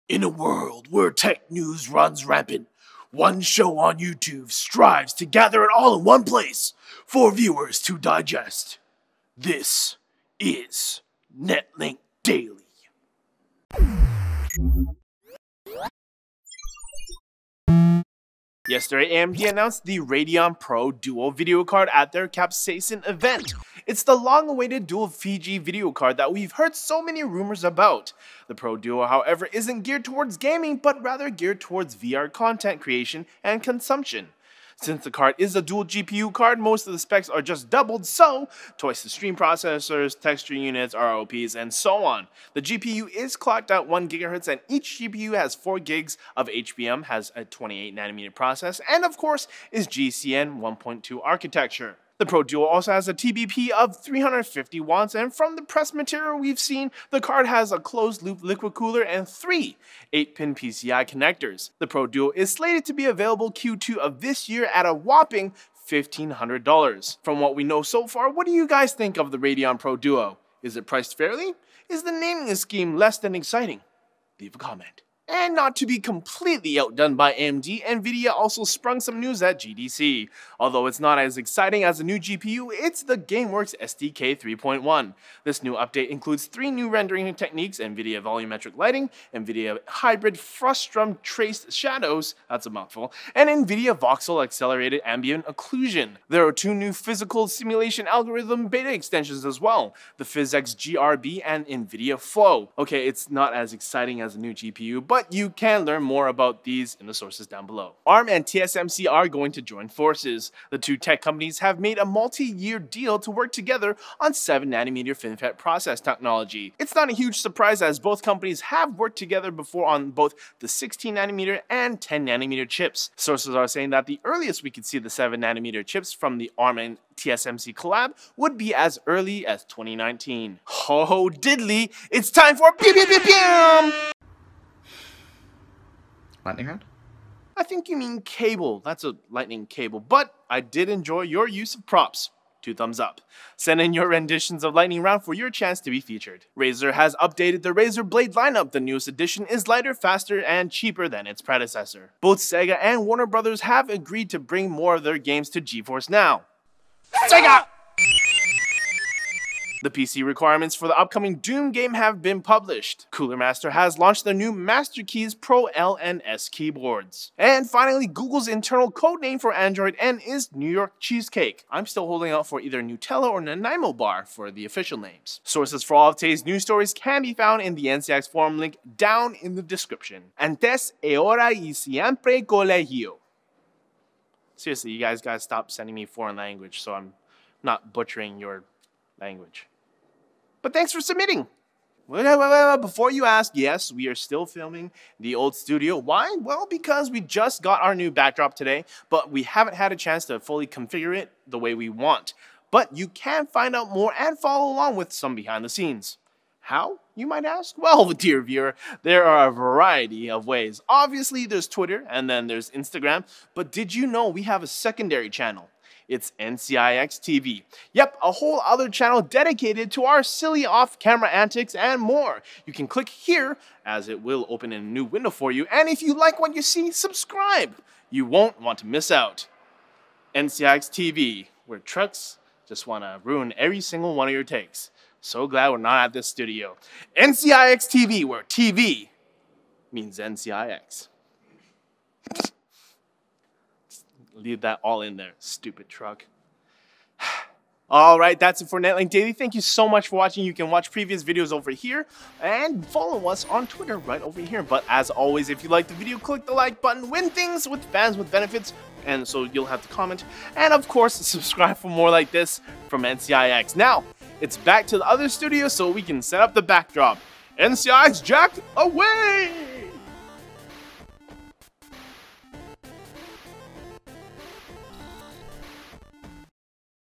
AMD’s New Pro Duo GPU, NVIDIA Gameworks Update, 7nm from ARM & TSMC
AMD officially announces their new double Fiji GPU. NVIDIA updates their Gameworks SDK. ARM and TSMC collab again for 7nm FinFET process. All this plus Android cheesecake on this episode of Netlinked Daily.

Episode Credits:
Host: Jack Sui
Writer: Jack Sui
Editor: Barret Murdock